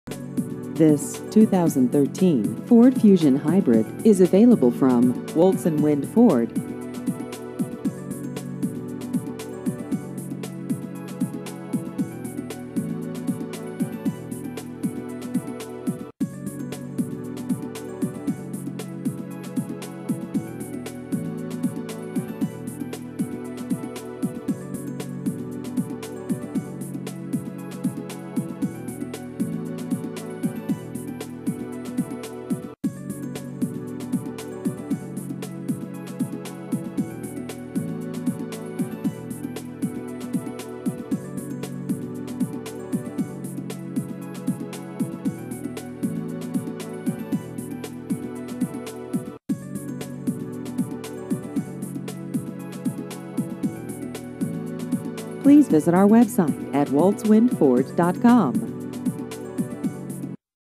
2013 Ford Fusion Hybrid New Cars Pittsburgh PA
This 2013 Ford Fusion Hybrid is available from Woltz & Wind Ford.